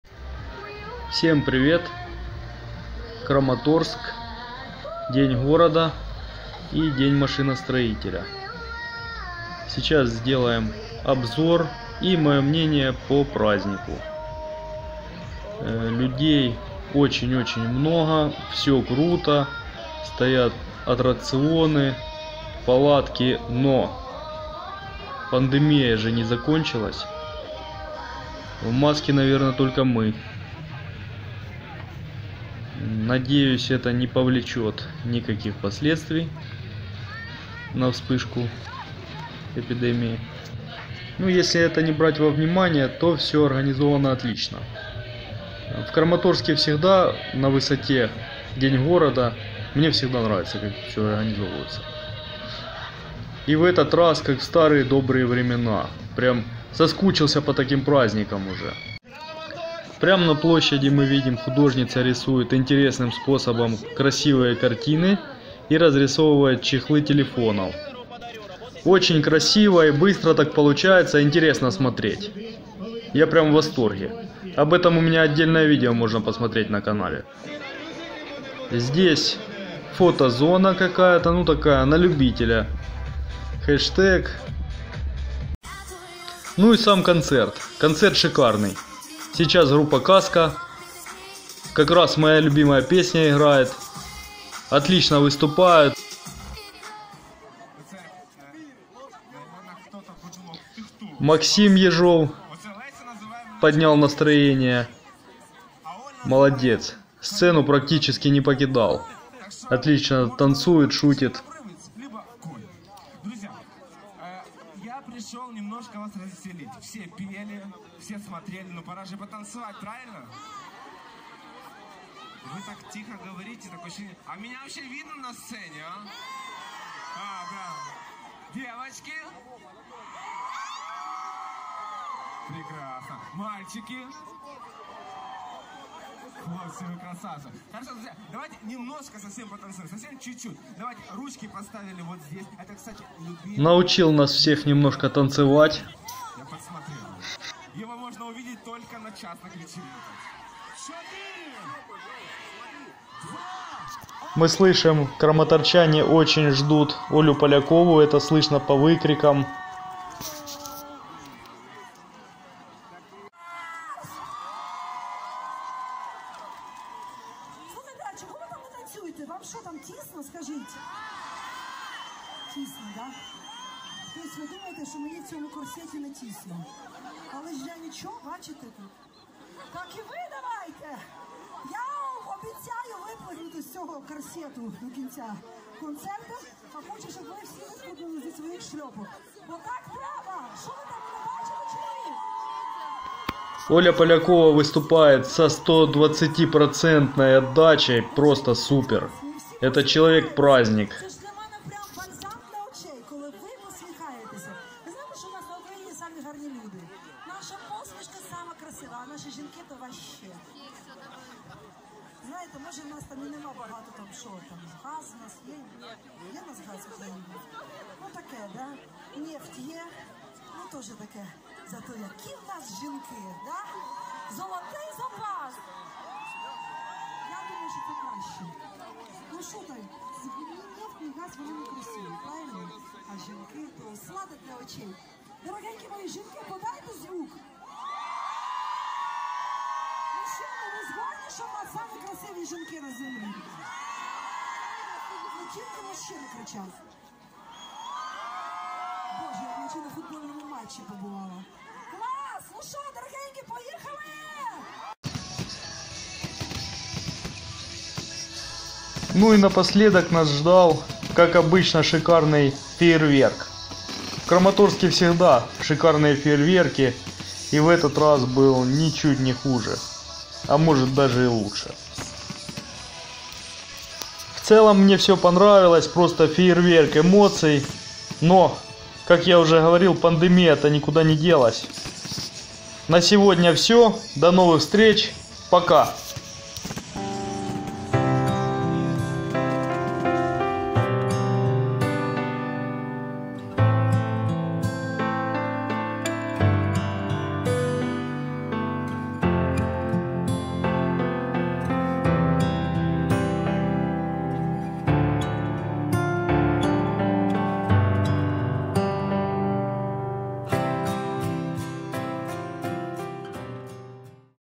Оля Полякова, Казка, Максим Ежов. День города, Краматорск
праздничный концерт, феерверк на день машиностроителя 2021 и день города Краматорск. Обзор организации